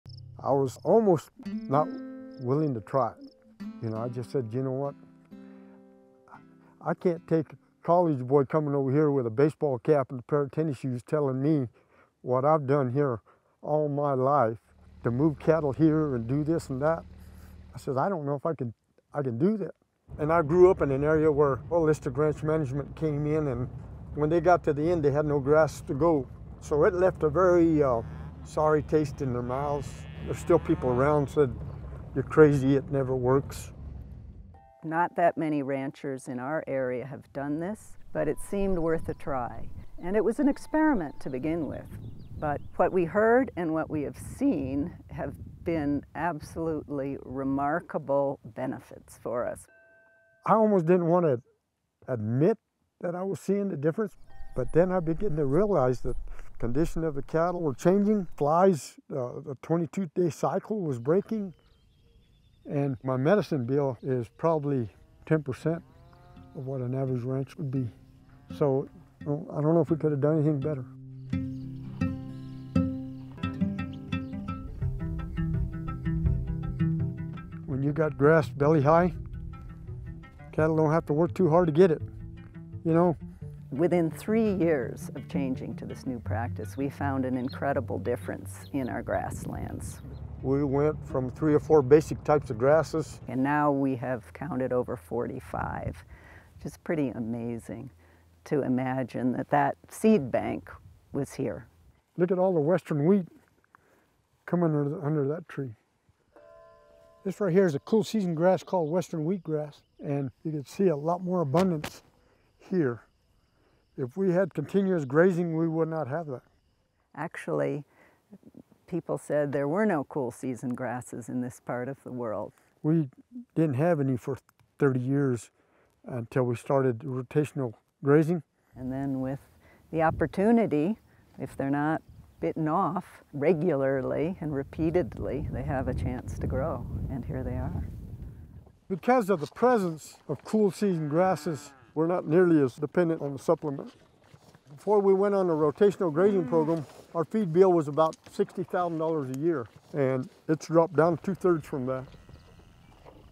He almost didn't try...
At the Ranney Ranch in arid Corona, New Mexico, Adaptive Multi-Paddock (AMP) grazing is restoring soils and benefiting the environment while producing healthy food for consumers – and they changed to AMP grazing during a 15-year drought – this was unheard of.  Ranch manager Melvin Johnson was extremely resistant to trying this new method of grazing, having been a conventional rancher all his life.
Filmed in Corona, New Mexico